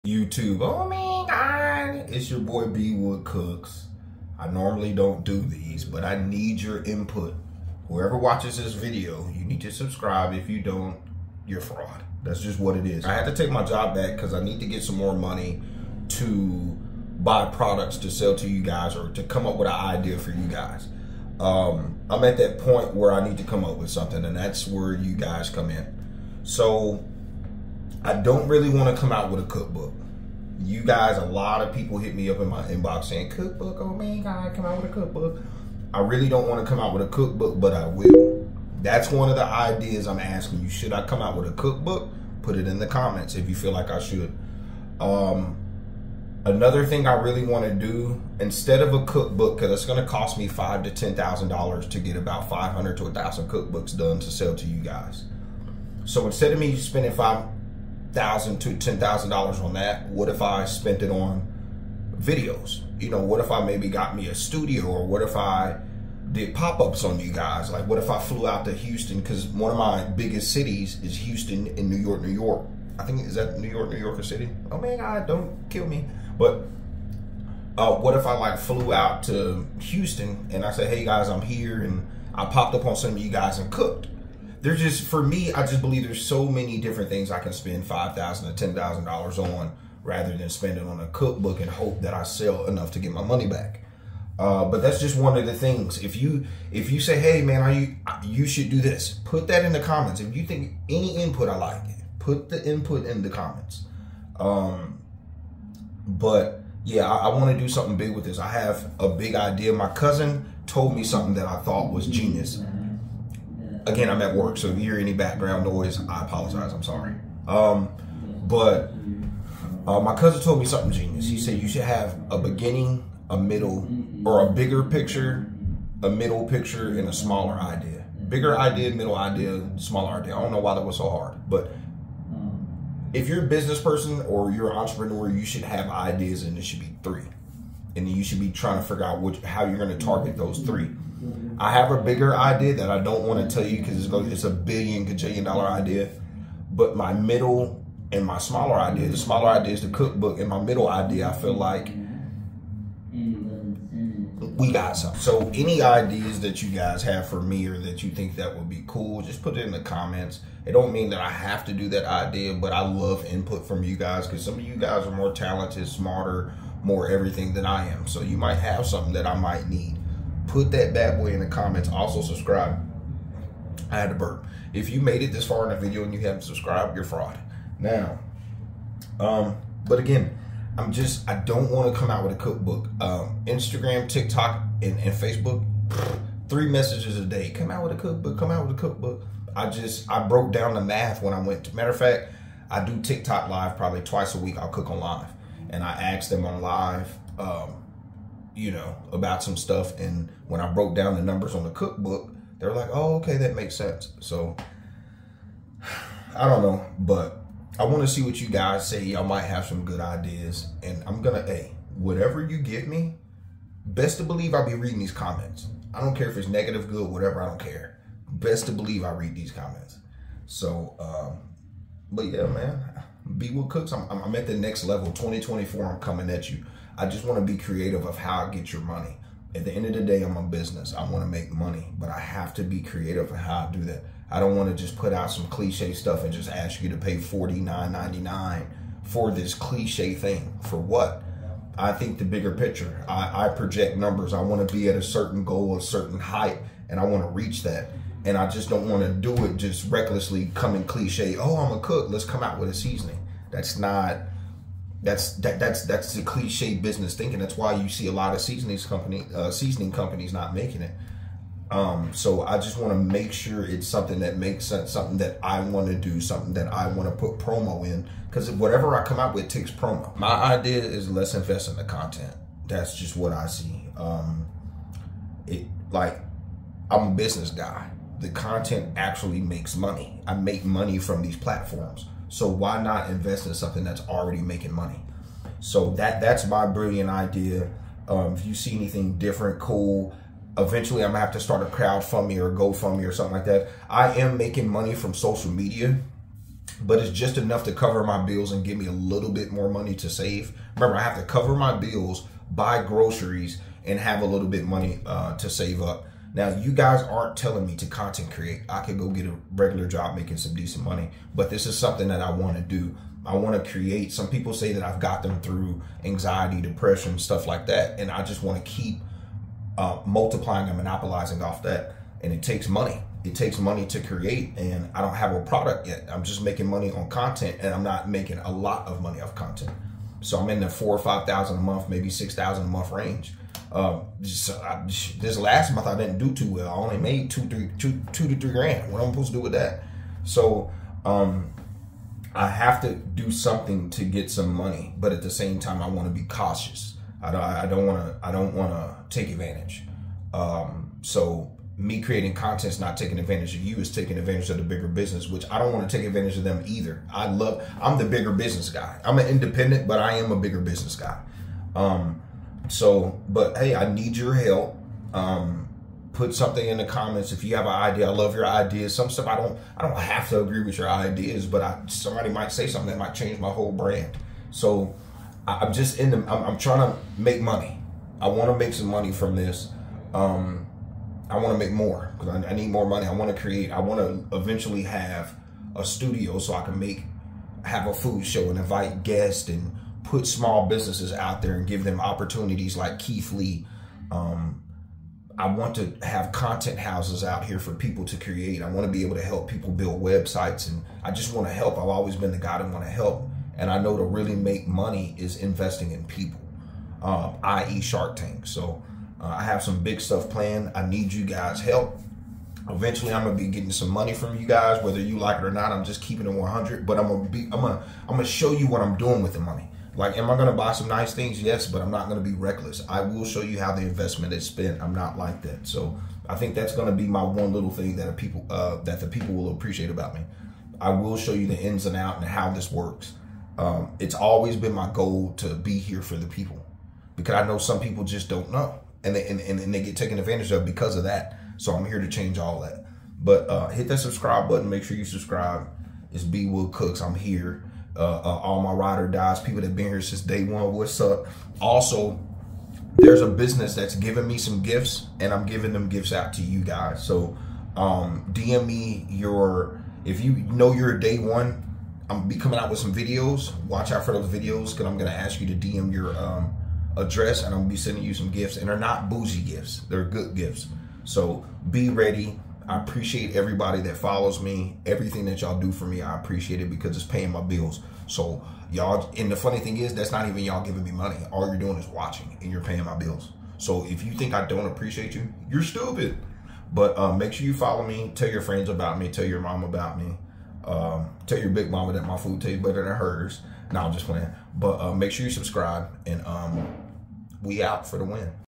Behind The Scenes: Making Ideas with You Guys
I'm currently earning from socialmedia but it's not enough to save or start a business. Not that yall have asked me to make bwoodcooks a business but I know it's bigger than me. I have $5-10k to create a product for you. Some suggest a cookbook, others say just create. I need help deciding what you would buy or whats a good idea or thing for me to generate cash. Let me know your preferences in the comments. I'll release more videos to gather data for the right product. Im Going hard in #2024 with three great ideas involving you. Don't want to go the cliché seasoning and cookbook route unless that's what you want. Comment your preferences For The Win.